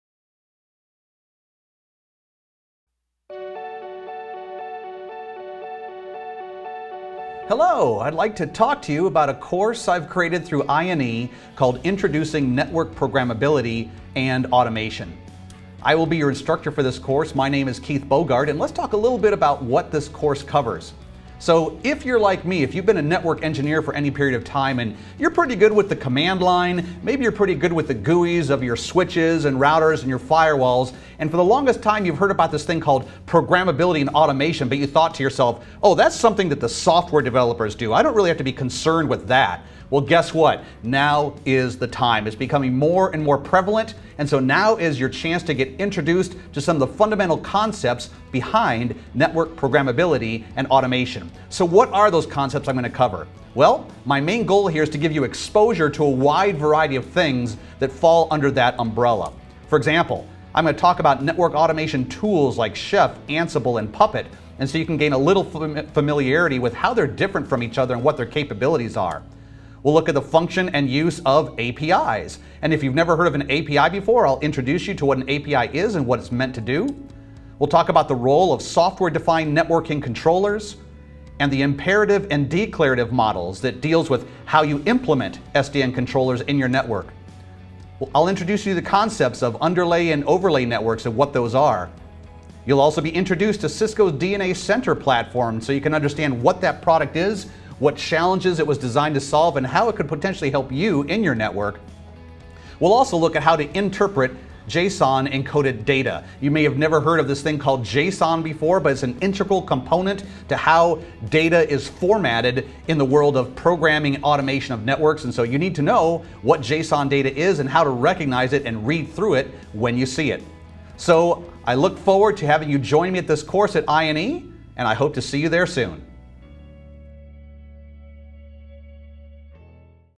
Introducing Network Programmability & Automation
Advance your networking knowledge with access to thousands of training videos instantly with INE's All Access Pass

This course will introduce you to the fundamental concepts and platforms that are involved with network automation and programmability, focused primarily on Cisco's approach and solution. Starting with an overview of the traditional methods of network management (and their challenges) we'll then progress through such topics as the goals of automation, common languages, tools and protocols used for automation, the imperative and declarative approaches to SDN implementations, how SDN provides the ability to separate the Control Plane from the Data Plane, an introduction to Cisco's DNA Center solution, an introduction to APIs with emphasis placed on REST APIs, overviews of Ansible, Chef, and Puppet as well as a whole lot more.